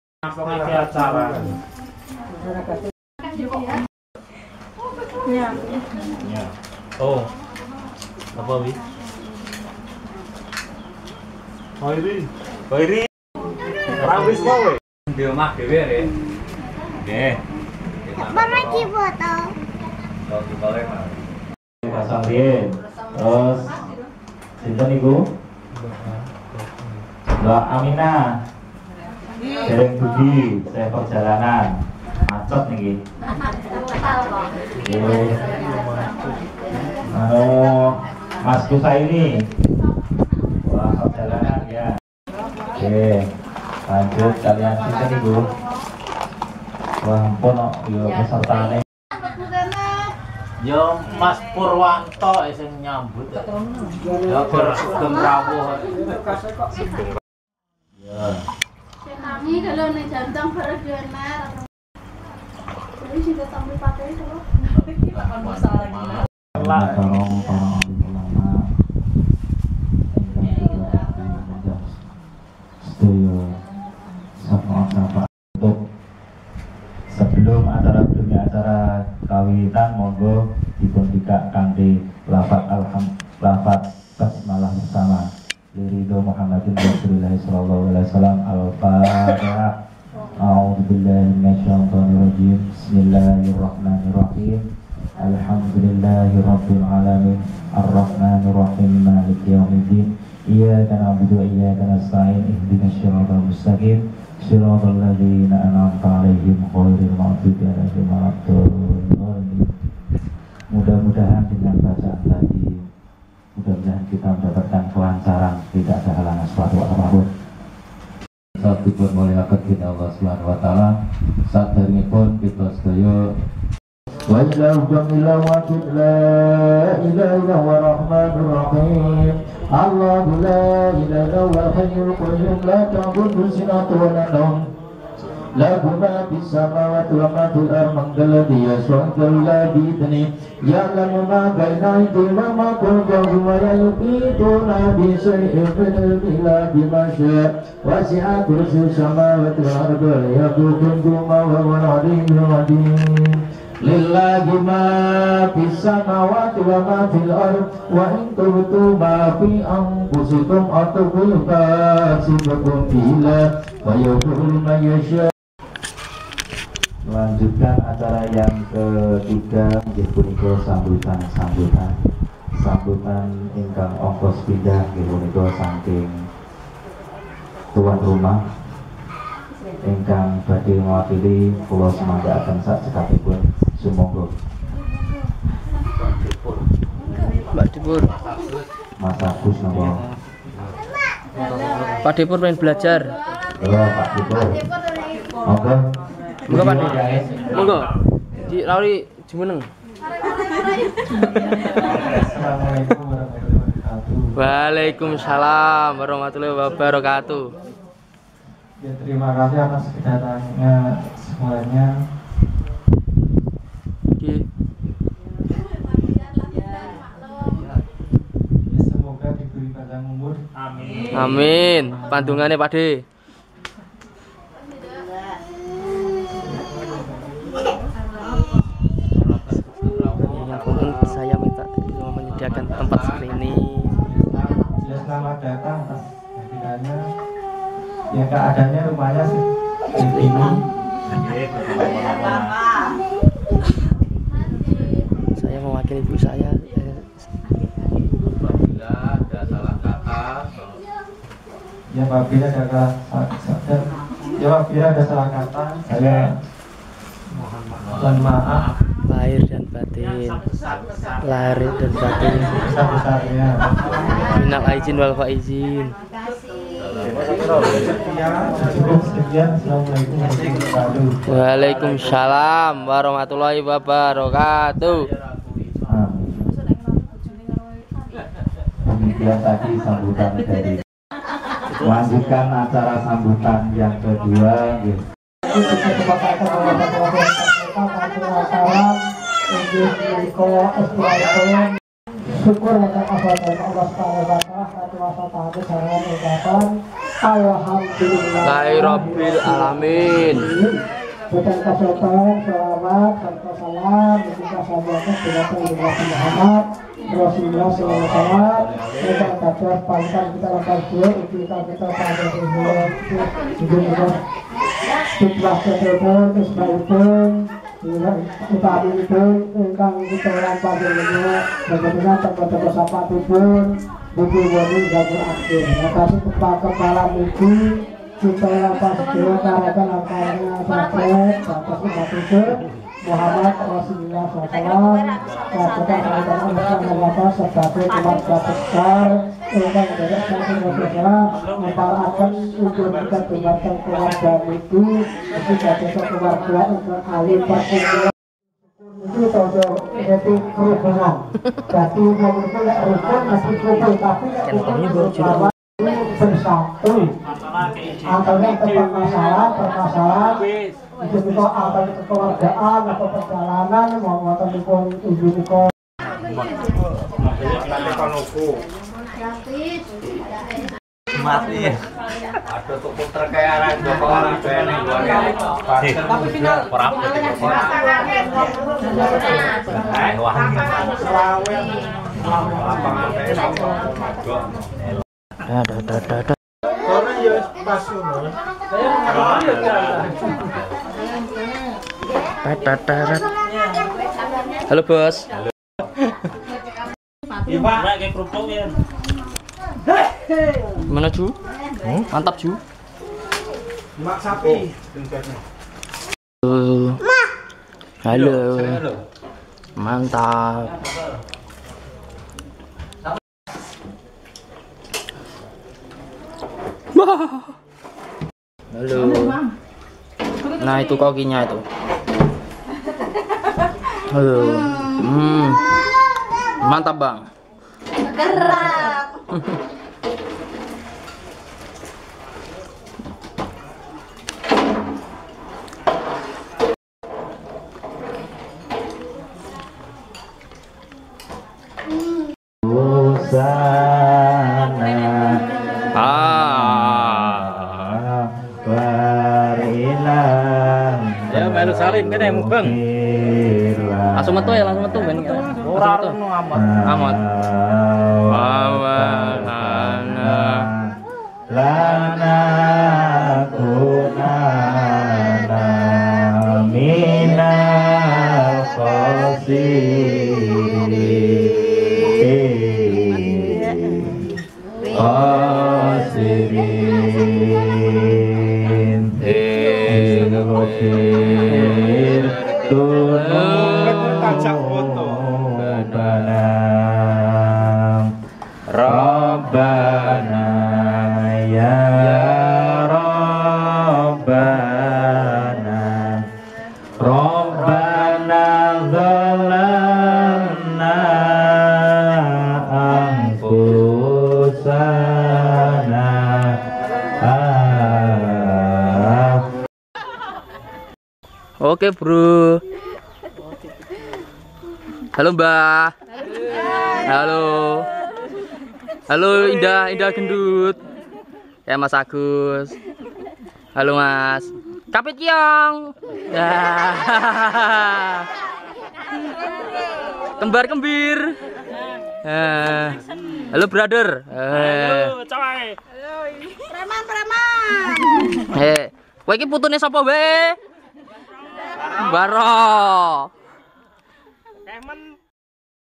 Arisan Keluarga Mbah Pawiro Usai Lebaran Idhul Fitri 2018
Dusun Karang, Desa Gading, Kecamatan Tanon, Kabupaten Sragen, Provinsi Jawa Tengah, Indonesia .

Keluarga
1. Ibu Tarni
2. Bapak Dirjo Wiranto
3. Parjan
4. Ngadimen
5. Suwarji
6.  Sumini